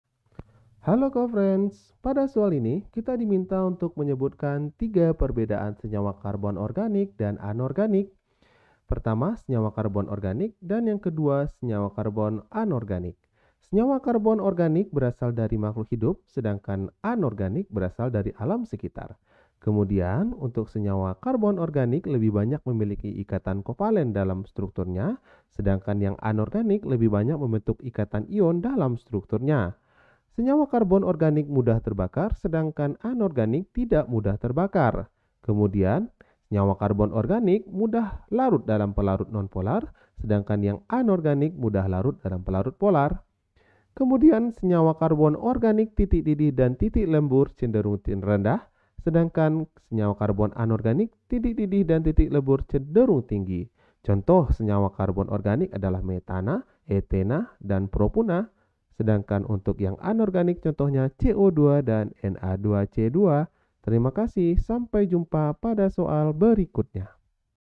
Sebutkan 3 perbedaan senyawa karbon organik dan anorganik:
Topik atau Materi: Kekhasan Atom Karbon - Senyawa Hidrokarbon - Kimia Organik - Kimia Kelas 11 SMA - KTSP

Sumber Soal: Latihan Mandiri Kimia Kelas 10, KTSP, SK, 2007 , N/A

00:00 : Bedah Soal
01:00 : Konsep, rumus dan pengertian Kekhasan Atom Karbon
01:18 : Langkah penyelesaian soal
00:07 : Jawaban akhir
00:37 : Penutup